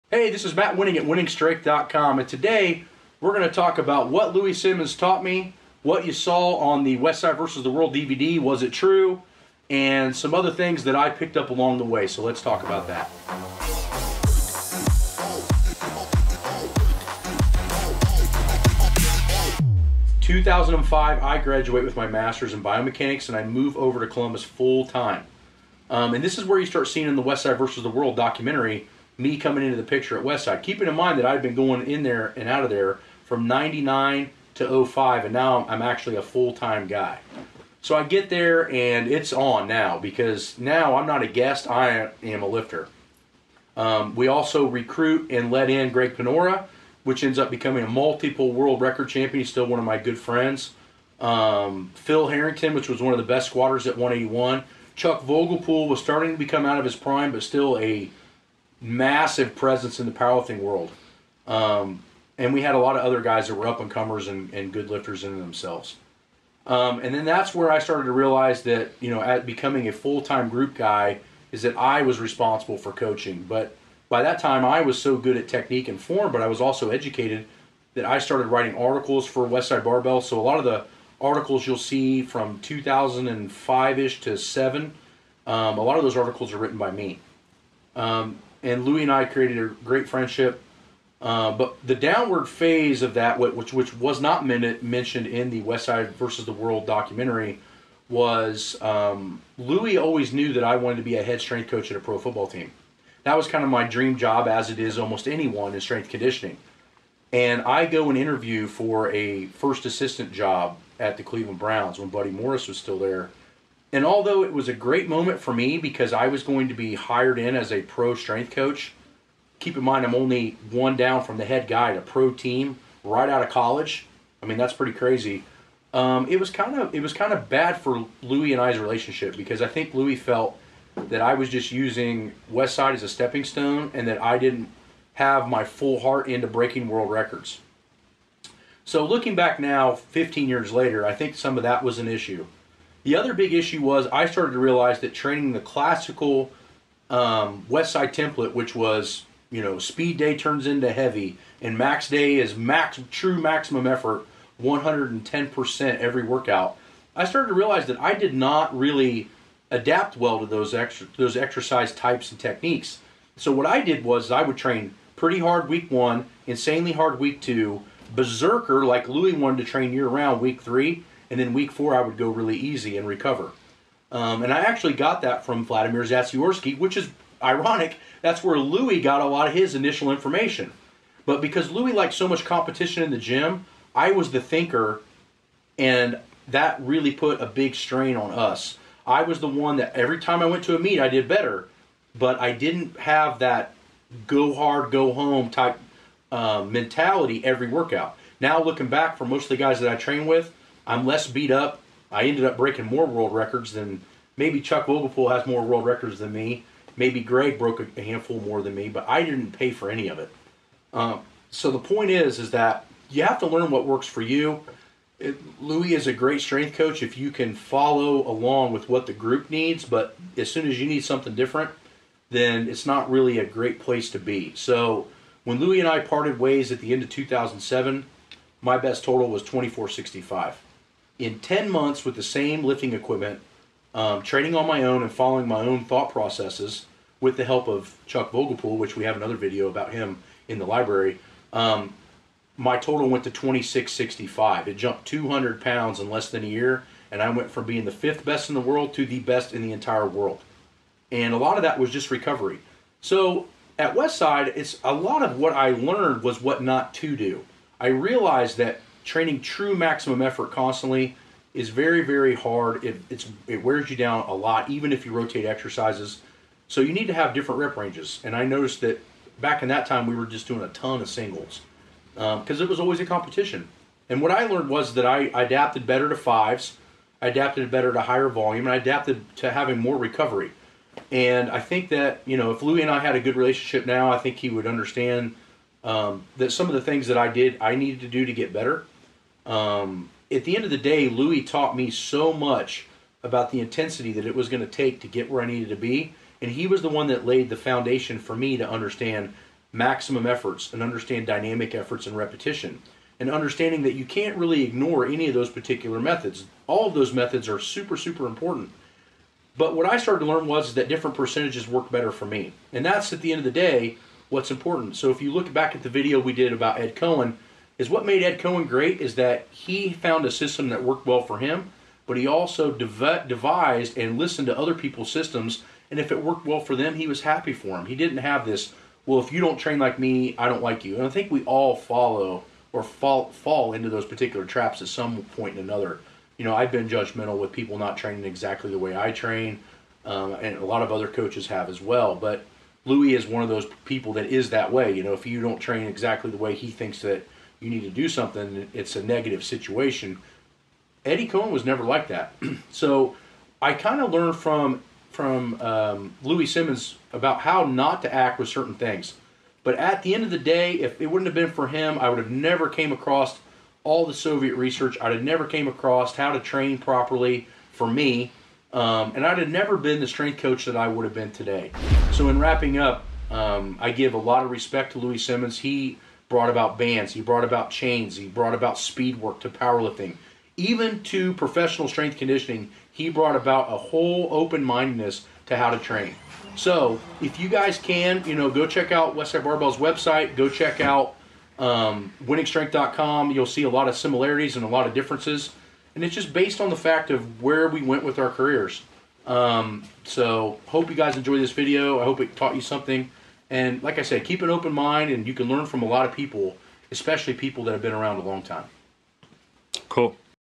My stories from Westside Barbell (Biggest things I learned!) | PART 2
Matt Wenning talks about his time at Westside Barbell (part 2)

Want to take your Powerlifting seriously? Grab the Powerlifting Training Manual to get step by step training.